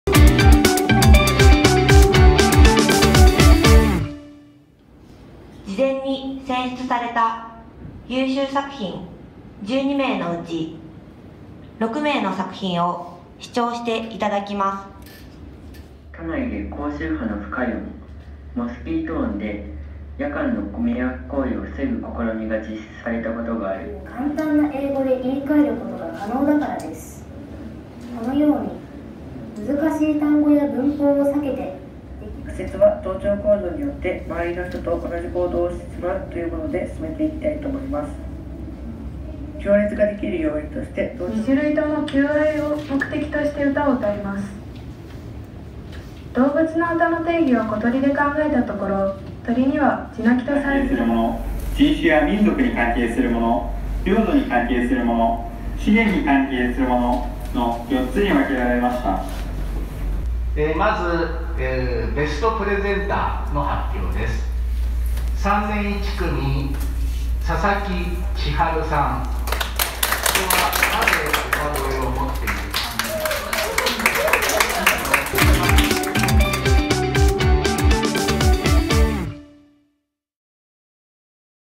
【千葉明徳中学校】2020年度 課題研究論文発表会ダイジェスト
2020年度 千葉明徳中学校 第３学年 課題研究論文発表会の様子です。
具体的な発表スライド動画も公開予定です。